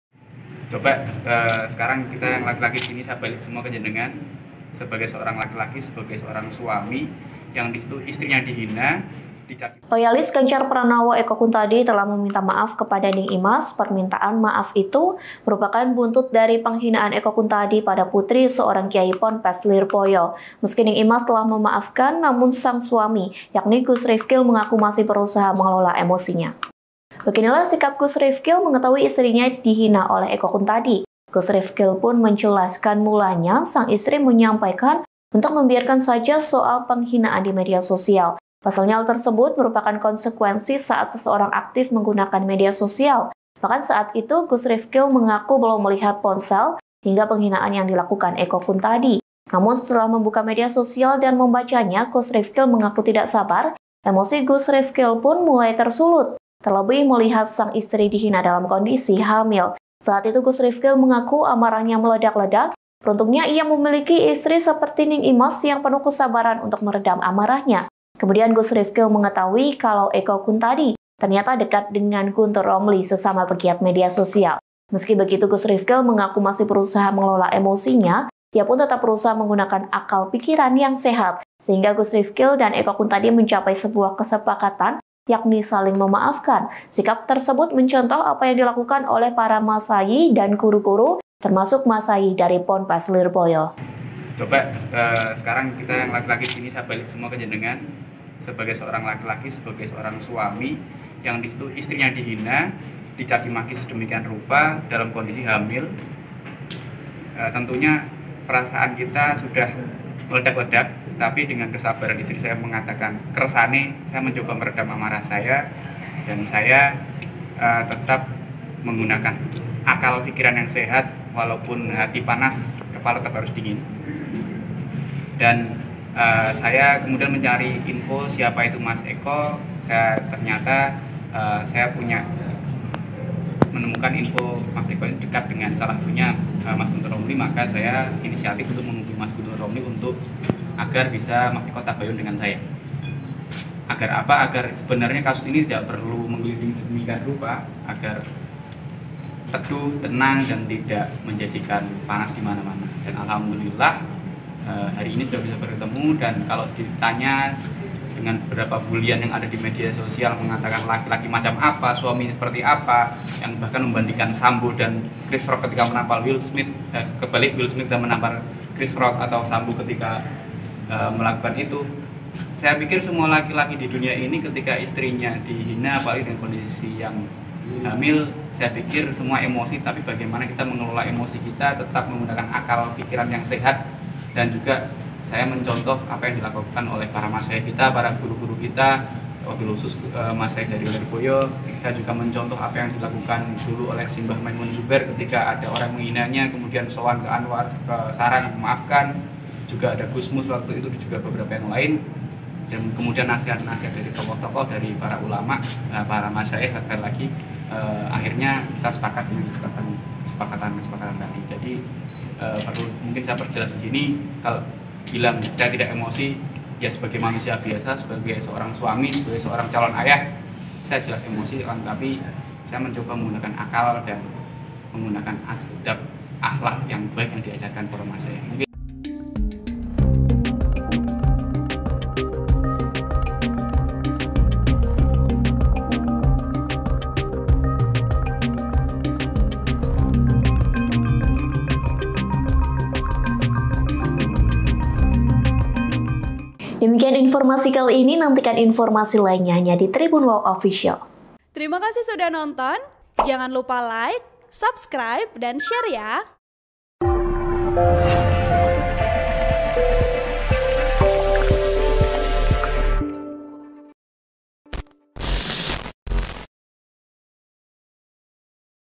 Sikap Suami Ning Imaz pada Eko Kuntadhi Tahu Istrinya Dihina saat Hamil, Buat Kesepakatan
TRIBUN-VIDEO.COM - Loyalis Ganjar Pranowo Eko Kuntadhi telah meminta maaf kepada Ning Imaz.

Permintaan maaf itu merupakan buntut dari penghinaan Eko Kuntadhi pada putri seorang Kiai Ponpes Lirboyo.

Meski Ning Imaz telah memaafkan, namun sang suami yakni Gus Rifqil mengaku masih berusaha mengelola emosinya.

Beginilah sikap Gus Rifqil mengetahui istrinya dihina oleh Eko Kuntadhi.

Bahkan saat itu, Gus Rifqil mengaku belum melihat ponsel hingga penghinaan yang dilakukan Eko Kuntadhi.

"Mas kersane, biarkan saja. Itu resiko bermedia sosial. Awalnya itu. Saya belum melihat HP, belum melihat apapun," ungkapnya.

Emosi Gus Rifqil pun mulai tersulut terlebih melihat sang istri dihina dalam kondisi hamil.

"Coba sekarang kita yang laki-laki, kita balik ke jenengan, sebagai seorang laki-laki, sebagai seorang suami, istrinya dihina dicaci maki sedemikian rupa dalam kondisi hamil," ungkapnya.

Saat itu, Gus Rifqil mengaku amarahnya meledak-ledak.

Beruntungnya ia memiliki istri seperti Ning Imaz yang penuh kesabaran untuk meredam amarahnya.

Meski begitu, Gus Rifqil mengaku masih berusaha mengelola emosinya.

Ia pun tetap berusaha menggunakan akal pikiran yang sehat.

Sehingga Gus Rifqil dan Eko Kuntadhi mencapai sebuah kesepakatan yakni saling memaafkan.

Sikap tersebut mencontoh apa yang dilakukan oleh para masayih dan guru-guru.

Termasuk masayih dari Ponpes Lirboyo.

"Saya juga mencontoh apa yang dilakukan oleh KH Maemun Zubair ketika ada orang yang menghinanya kemudian sowan dan memaafkan, juga contoh dari Gus Mus dan nasehat tokoh -tokoh para ulama. Akhirnya kita sepakat (memaafkan)," ungkapnya.

(Tribun-Video.com/Surya.co.id)

Artikel ini telah tayang di Surya.co.id dengan judul Maafkan Eko Kuntadhi, Ning Imaz Ingatkan untuk Berhati-hati dalam Bermedia Sosial,

Video Production: Nabila A Fadhila